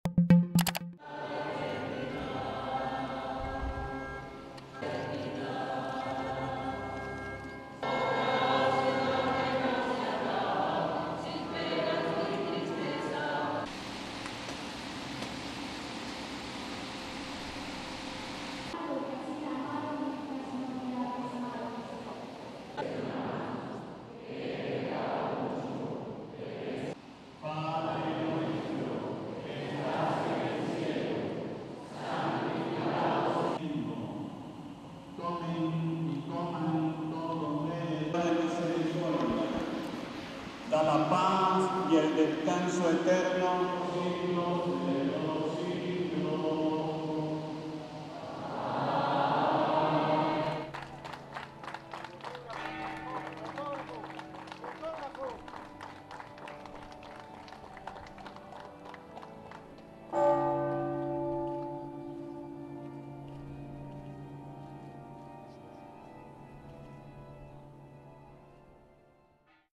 Cubans bid farewell to Cardinal Ortega, late leader of the island’s Catholics
Havana (Cuba), Jul 28 (EFE).- Cuban Catholics on Sunday bade farewell to Cardinal Jaime Ortega, the leader of the island's Church, in a funeral mass held at the Cathedral of Havana attended by the faithful, bishops and priests as well as representatives of the Cuban government.

POOL FOOTAGE.